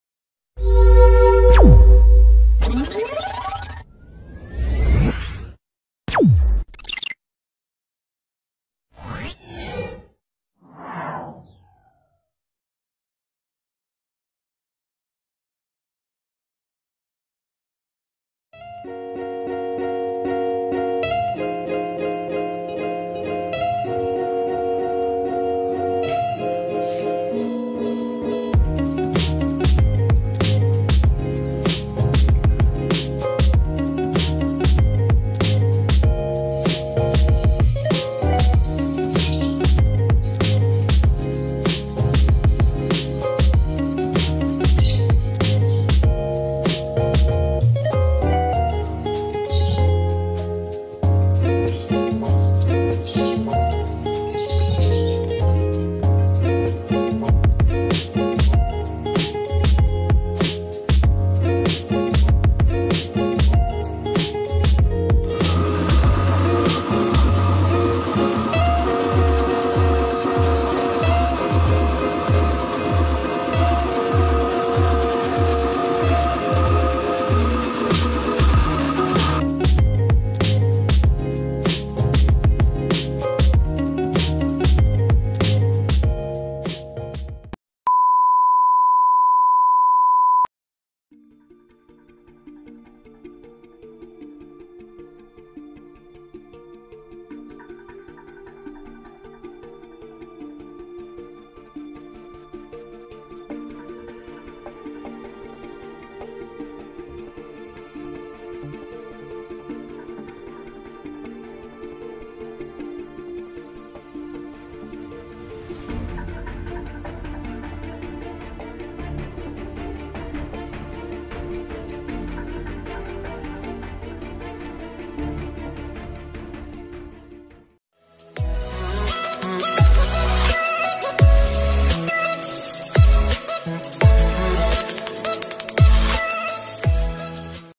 REPAINT/CAT ULANG VELG RACING MOBIL
Video singkat cara repaint/Cat ulang velg racing mobil dari warna awalnya hitam menjadi warna silver.

Painting Proses:
"LANA AIRBRUSH"/Singo-Singo

Batam-Kepri